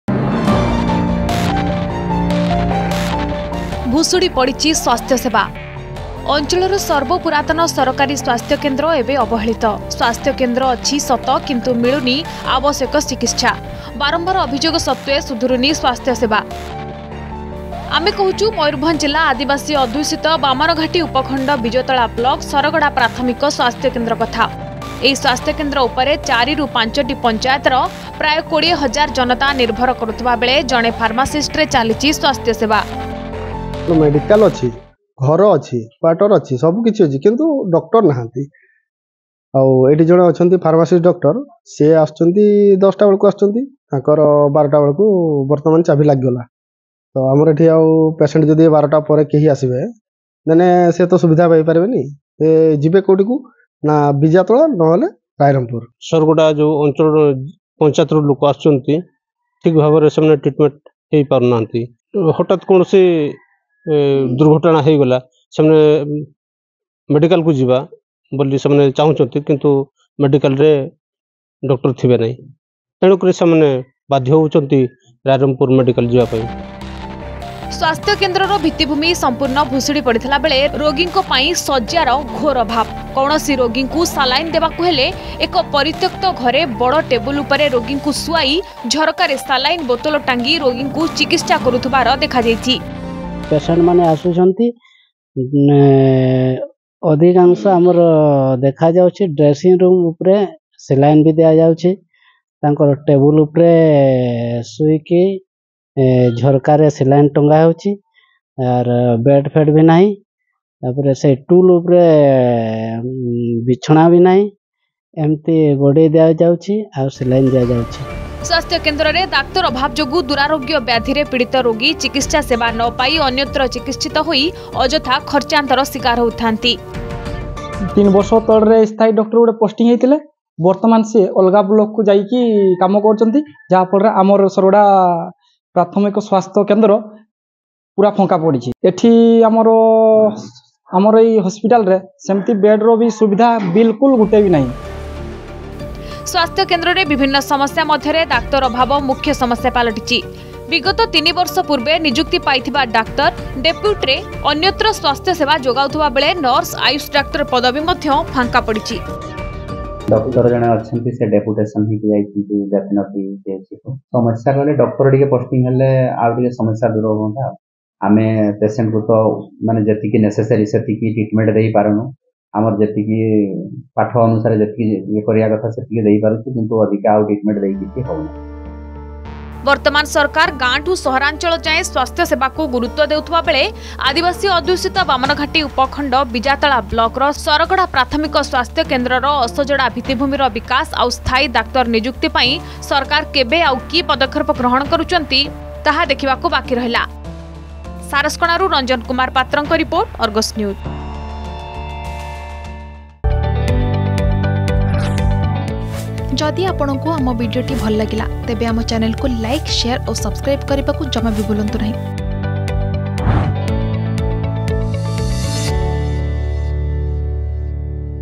ଫାରମାସିଷ୍ଟ ଭରଷାରେ ଚାଲିଛି ପ୍ରାଥମିକ ସ୍ବାସ୍ଥ୍ୟ କେନ୍ଦ୍ର || Saragada || Mayurbhanj
ଅଞ୍ଚଳର ସର୍ବପୁରାତନ ସରକାରୀ ସ୍ବାସ୍ଥ୍ୟ କେନ୍ଦ୍ର ଏବେ ଅବହେଳିତ। ସ୍ବାସ୍ଥ୍ୟ କେନ୍ଦ୍ର ଅଛି ସତ କିନ୍ତୁ ମିଳୁନି ଆବଶ୍ୟକ ଚିକିତ୍ସା, ବାରମ୍ବାର ଅଭିଯୋଗ ସତ୍ତ୍ବେ ସୁଧୁରୁନି ସ୍ବାସ୍ଥ୍ୟ ସେବା।